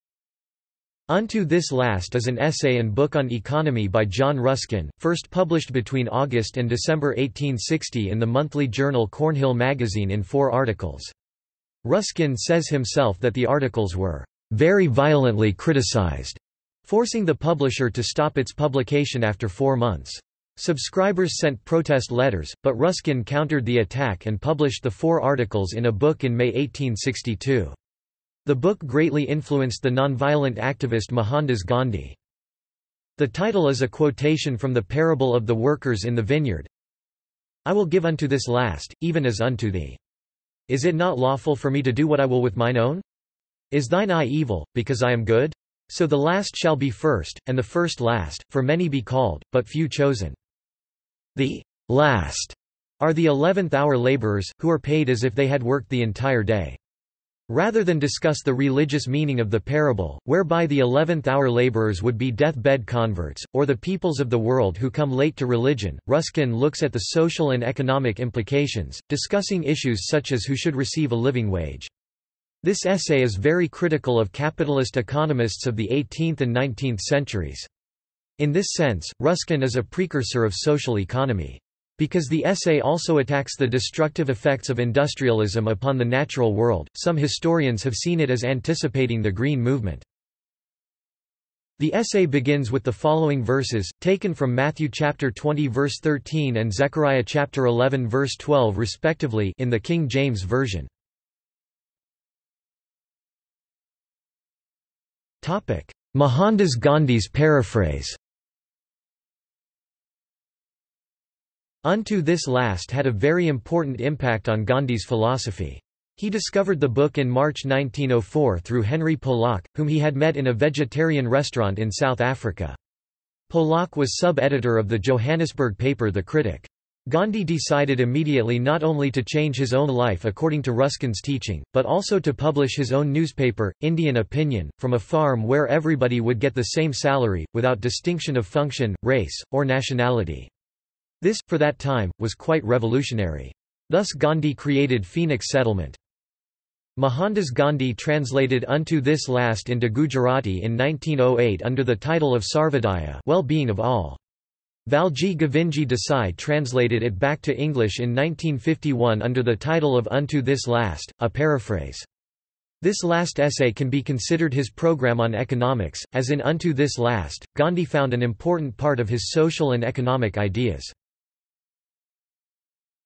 Unto This Last | Wikipedia audio article
This is an audio version of the Wikipedia Article:
Unto This Last

00:01:41 undefined

- Socrates

SUMMARY
Unto This Last is an essay and book on economy by John Ruskin, first published between August and December 1860 in the monthly journal Cornhill Magazine in four articles. Ruskin says himself that the articles were "very violently criticized", forcing the publisher to stop its publication after four months. Subscribers sent protest letters, but Ruskin countered the attack and published the four articles in a book in May 1862. The book greatly influenced the nonviolent activist Mohandas Gandhi.
The title is a quotation from the Parable of the Workers in the Vineyard:

I will give unto this last, even as unto thee. Is it not lawful for me to do what I will with mine own? Is thine eye evil, because I am good? So the last shall be first, and the first last: for many be called, but few chosen.

The "last" are the eleventh hour labourers, who are paid as if they had worked the entire day.  Rather than discuss the religious meaning of the parable, whereby the eleventh hour labourers would be death-bed converts, or the peoples of the world who come late to religion, Ruskin looks at the social and economic implications, discussing issues such as who should receive a living wage. This essay is very critical of capitalist economists of the 18th and 19th centuries. In this sense, Ruskin is a precursor of social economy. Because the essay also attacks the destructive effects of industrialism upon the natural world, some historians have seen it as anticipating the Green movement.

The essay begins with the following verses, taken from Matthew 20:13 and Zechariah 11:12 respectively (in the King James Version):